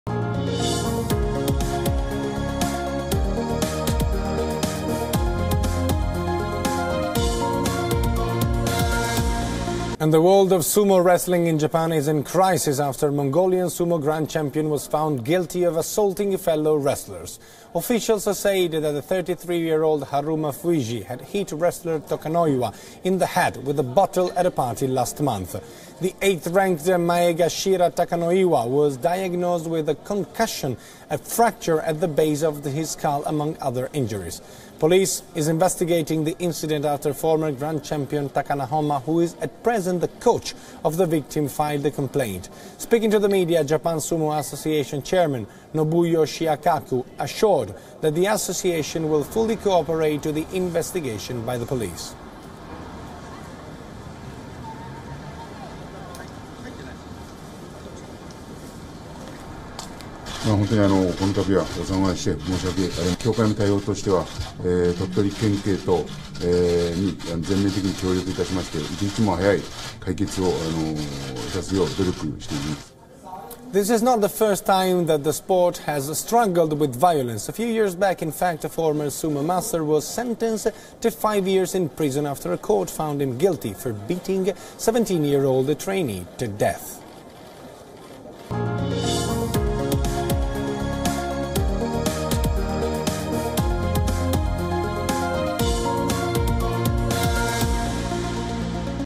Japan grapples with another Sumo scandal
Mongolian Sumo wrestler Yokozuna Harumafuji to face harsh penalty for beating junior. Former grand champion Takanohana filed a complaint.